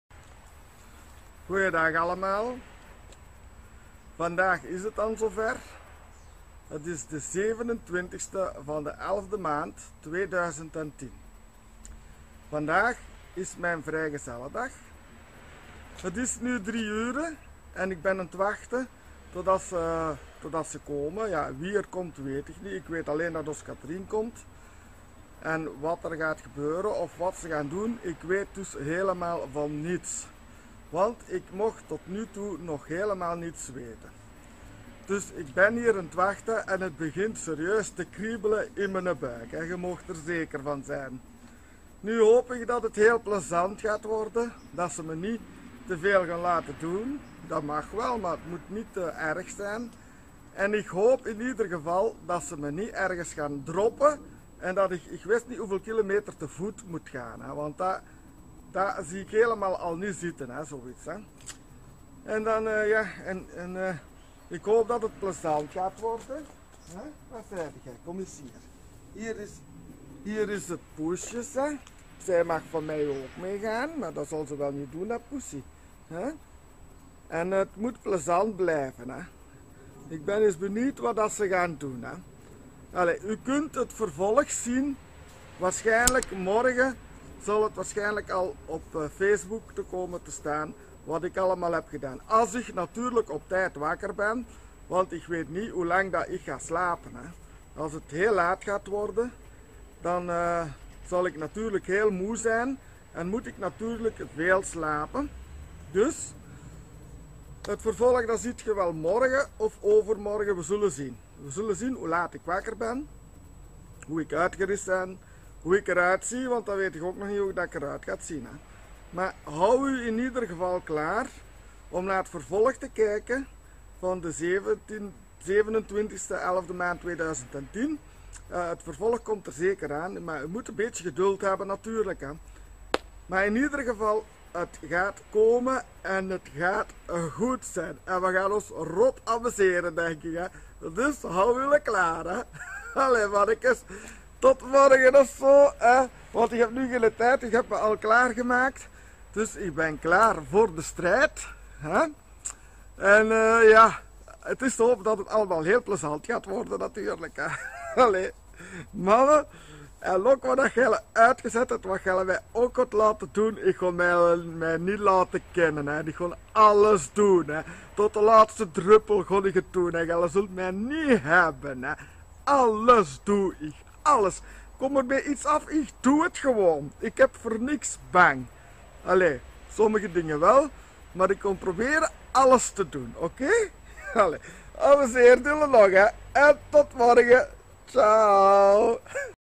27 11 2010 BEN AANT WACHTEN
MIJN VRIJGEZELLEDAG BEN AAN HET WACHTEN OP DE MENSEN DIE MIJ KOMEN HALEN OM TE VIEREN IK WEET NOG ALTIJD NIET WIE MEE GAAT ALS HET MAAR PLEZANT IS